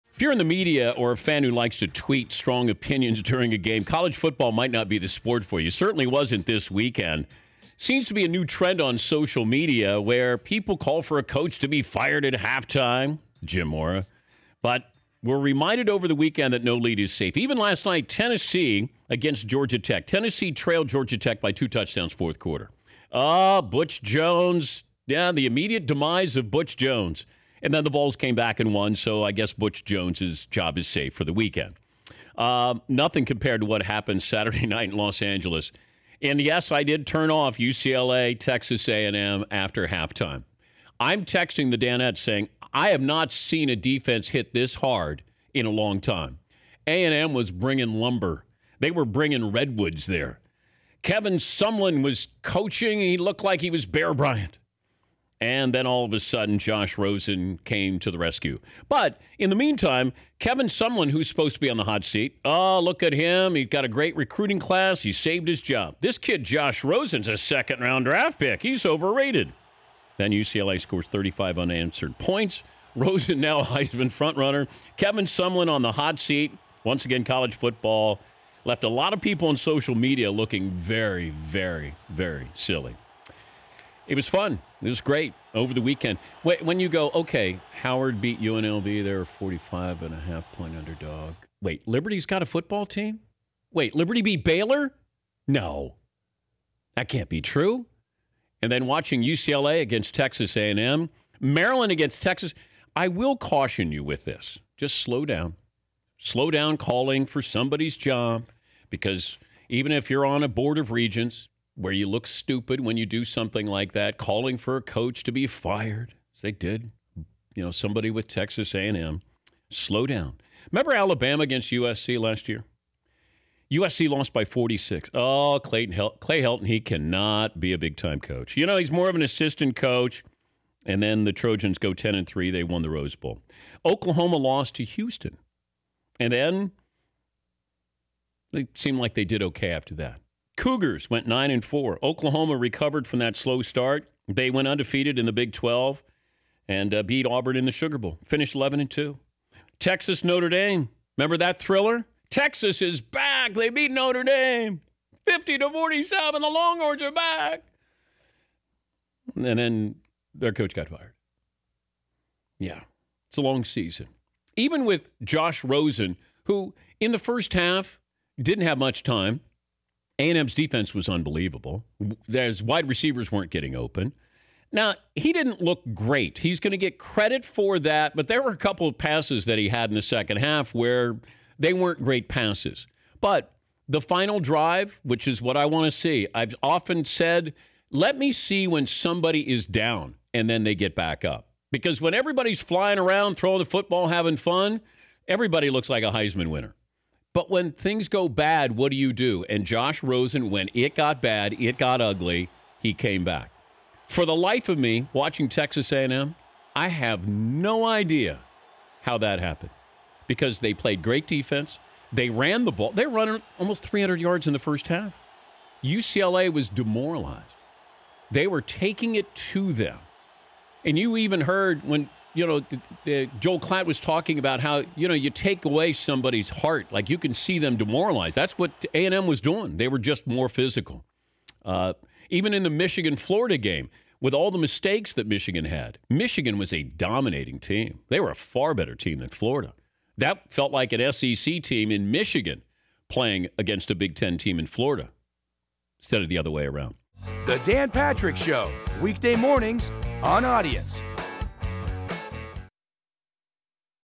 Dan Patrick Reacts to College Football's Wild Weekend | 9/5/17
The only thing wilder than the great finishes between UCLA vs Texas A&M, Tennessee vs Georgia Tech and others was the fans premature reactions on social media.  Dan Patrick weighs in on both.